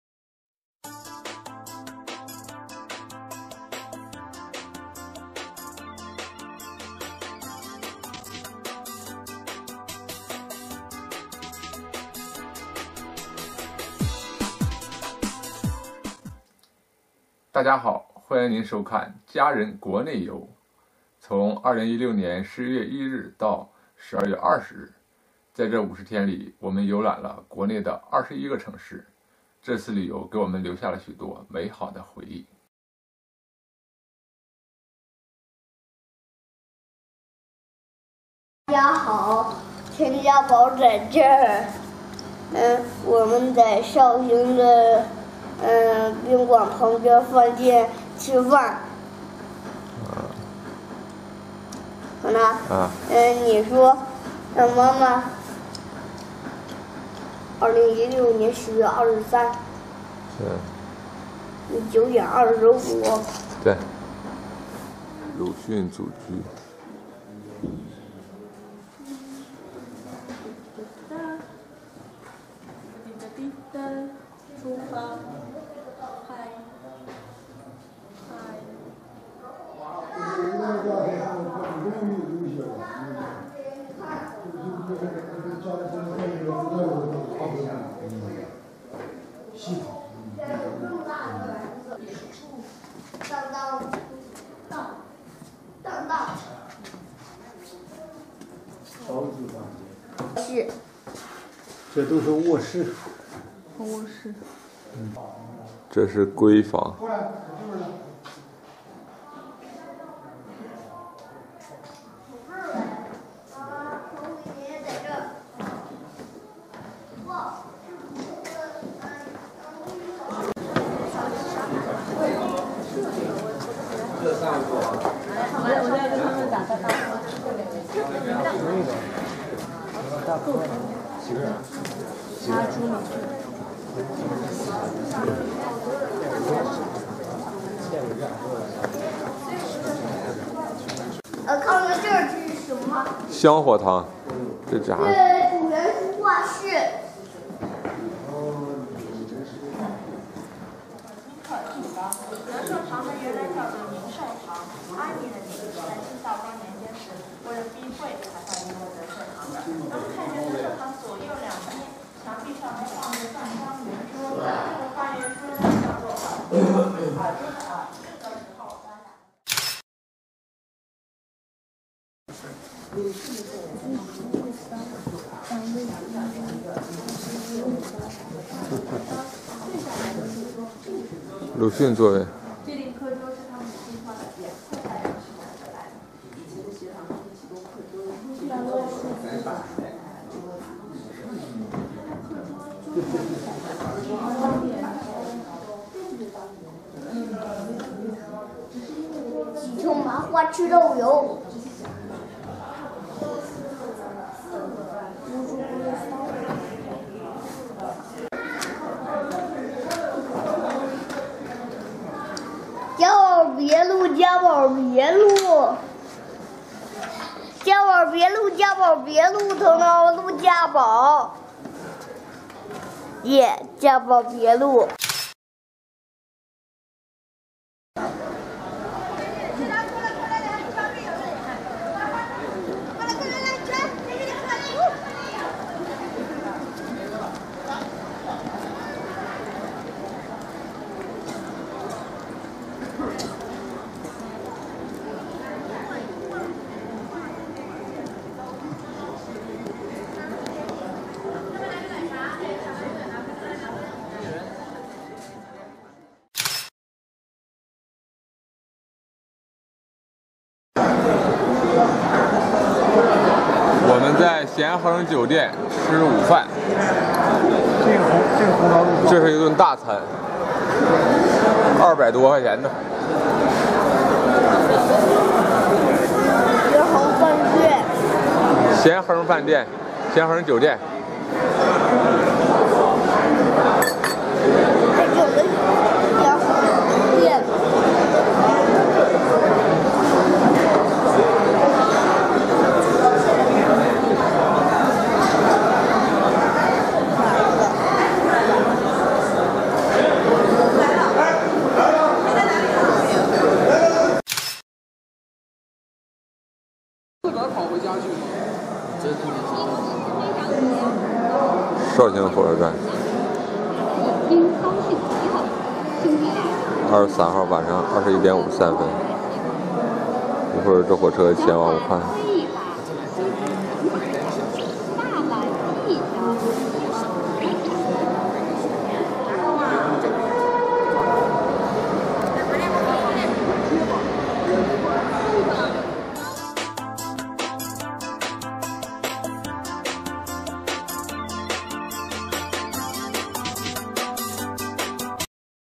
俄语导游翻译陈彤 家人国内游 11 绍兴Russian tour guide Chen Tong family travel shaoxing
Russian tour guide Chen Tong family travel2016年11月1日至12月20日，我和家人游览了中国的25个城市。很开心。
2016年12月20日から2016年11月1日、私の家族と私は、中国の25都市を見学。非常に満足。
From November 1 to December 20, 2016, my family and I visited 25 cities in China. Very happy.
2016  1 ноября по 20 декабря, я и моя семья гастролировали Китай 25 городов. Очень рад.